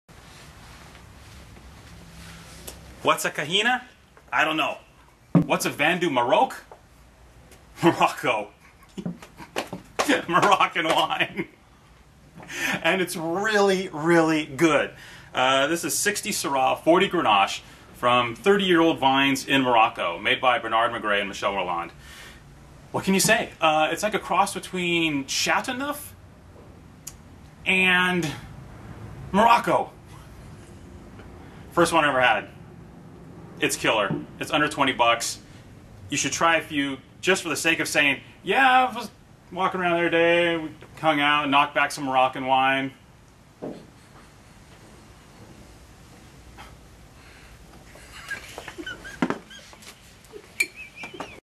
Best Wines Online: Kahina Morocco 2010
Not a typo. Kyle discovers some kickin' Moroccan from Kahina!